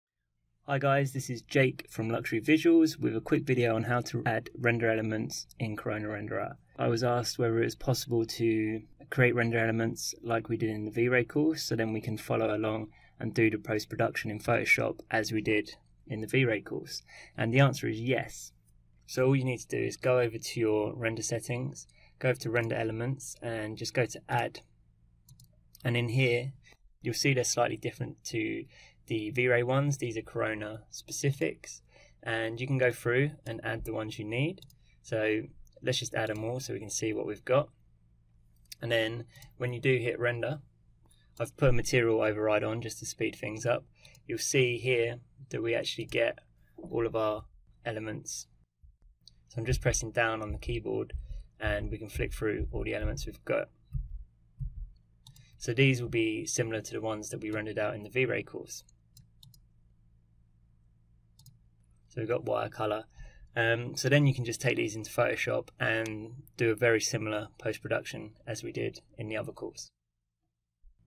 How to add Corona Render Elements in 3ds Max
In this video I show how to add render elements like the ones in Vray. By doing this we can follow a similar post-production workflow as the Vray classes.

Interior 3D Rendering with 3ds Max + Vray : The Quickest Way

Interior 3D Rendering 3ds Max + Corona : The Quickest Way